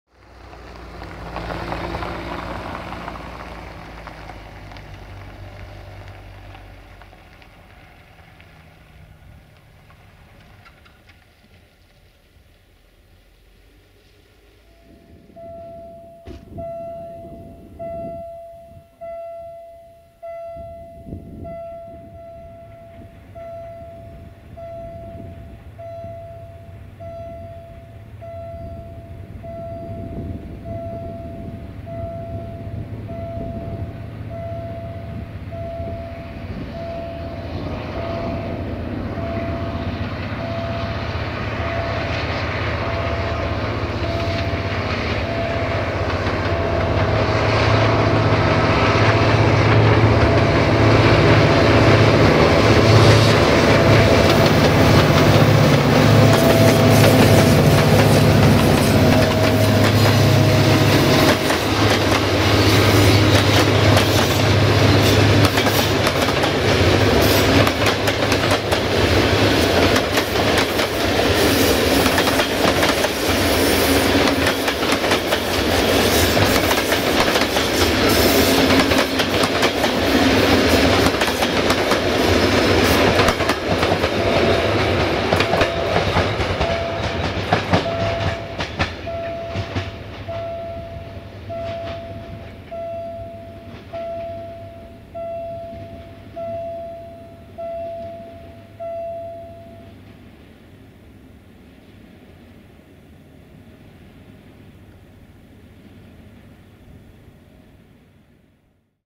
GM 65-0990-5 cu IR 1742 Satu Mare - Bucuresti Nord Demarand din Bratca in Noapte
GM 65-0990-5 with IR 1742 Satu Mare - Bucuresti Nord Accelerating from Bratca in Night

1:IR1742 - Satu Mare - Bucuresti Nord - Jimmy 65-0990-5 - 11 Vagoane/Carriages

© Train97